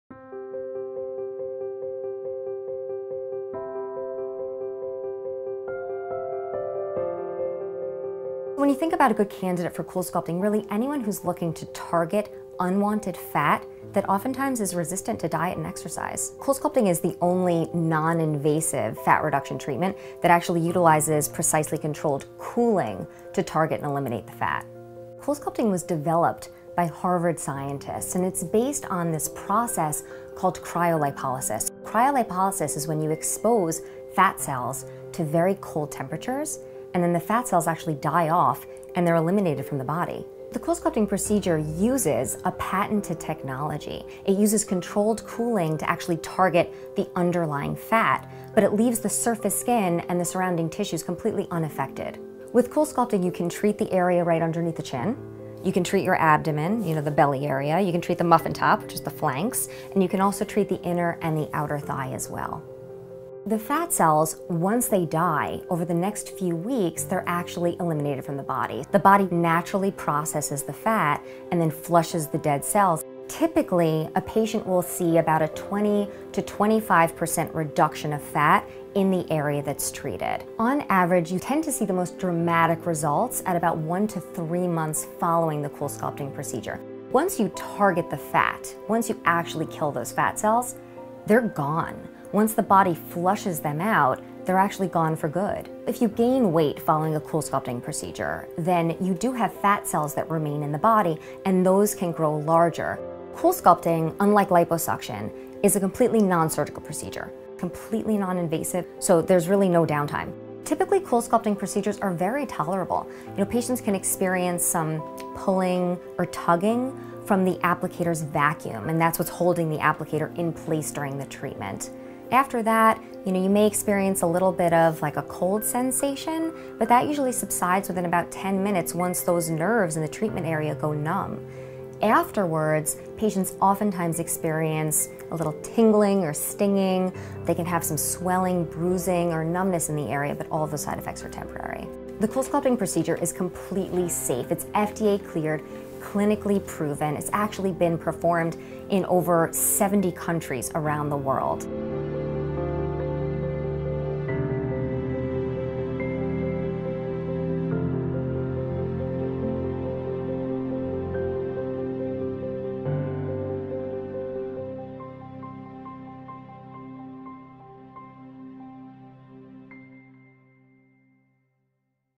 A Top Dermatologist Explains How CoolSculpting Really Works
CoolSculpting Freezes & Eliminates Stubborn Fat. The CoolSculpting procedure safely delivers precisely controlled cooling to gently and effectively target the fat cells underneath the skin. Treated fat cells are crystalized (frozen), then die.